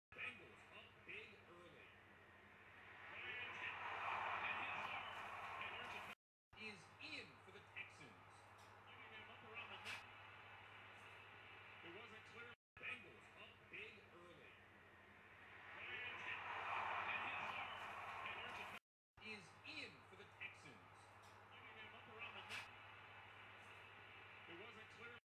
LOOK! Steelers LB destroys Matt Ryan, refs throw iffy roughing the passer flag
That's Steelers linebacker Jason Worilds destroying Ryan with what appears to be a clean hit. However, the refs didn't think it was cleaned and Worilds was flagged for roughing the passer